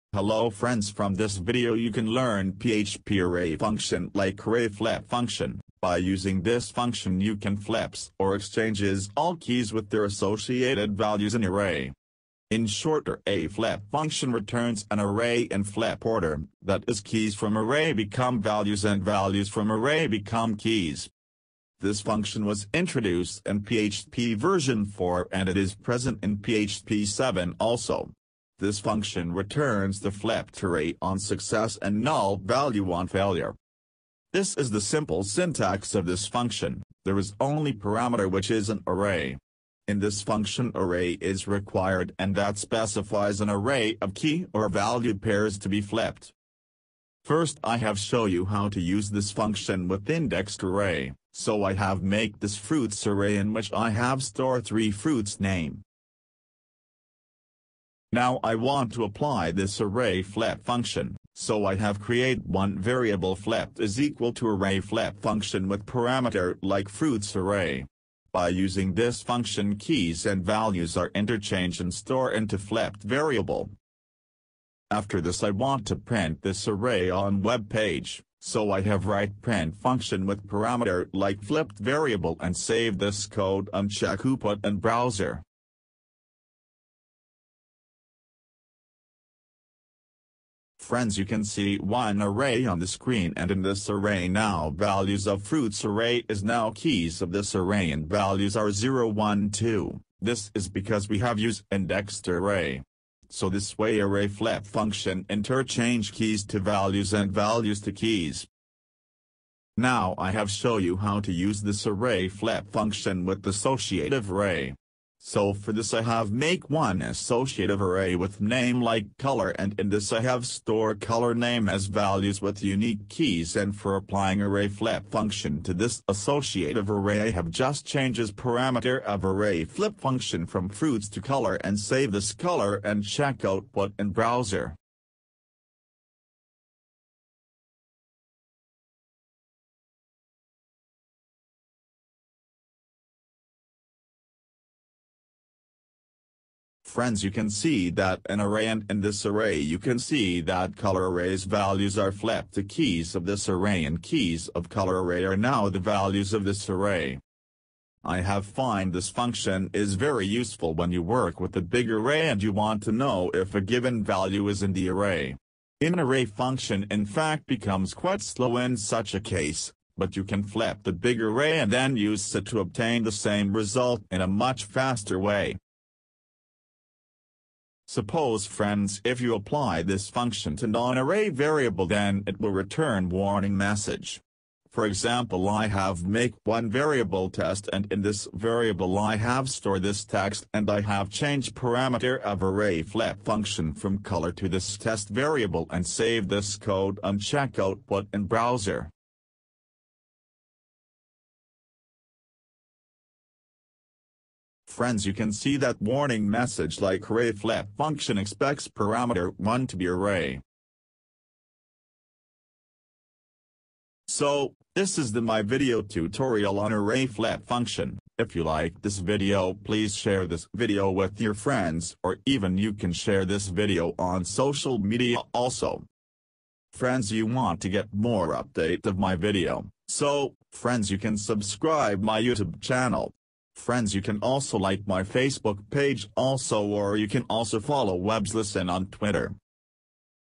PHP Array Functions - array_flip() Function
If you want to learn about how to exchanges values to keys and keys to values in php array, then you can learn array_flip() function from this video. This function you can flips or exchanges all keys with their associated values in array.